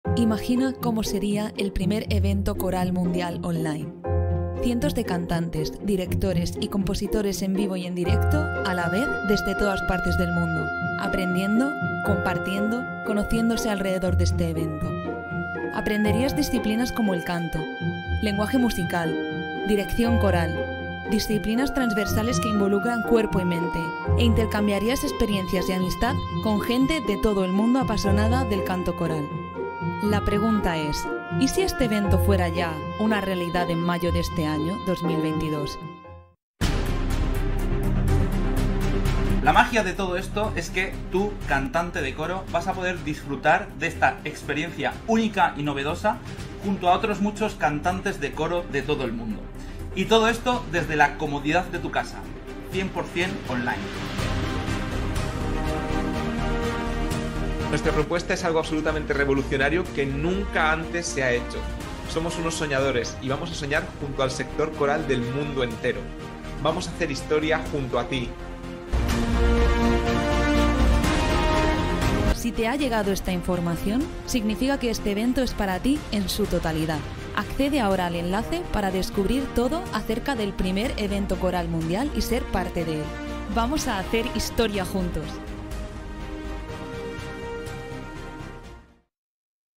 🚀 🚀 I EVENTO CORAL MUNDIAL ONLINE ¿Te lo vas a PERDER? 😱
El PRIMER EVENTO CORAL MUNDIAL Online en habla hispana tendrá lugar este año, 2022. Vive una experiencia INOLVIDABLE junto a todo el mundo coral los días 13, 14 y 15 de mayo desde la comodidad de tu casa. Aprovecha que la entrada tiene un precio reducido por el momento. Accede al enlace para más información.